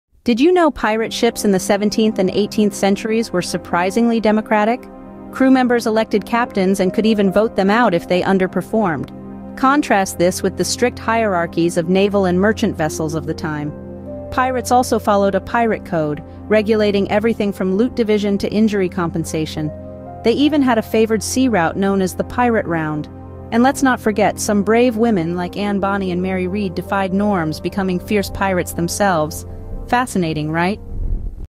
Pirate Democracy: A Voyage into the Past
In this thought-provoking video, we dive deep into the captivating world of pirate ships and explore the democratic principles that governed these fearless sailors. Join us as we unravel the mysterious code of conduct that bound pirate crews together and discover the thrilling tales of the infamous Pirate Round. From equality to consensus, we'll examine how these principles shaped the pirate way of life. So, grab your rum and set sail with us on this fascinating journey! Together, let's uncover the hidden treasures behind the democratic foundations of pirate ships. Ahoy, mateys!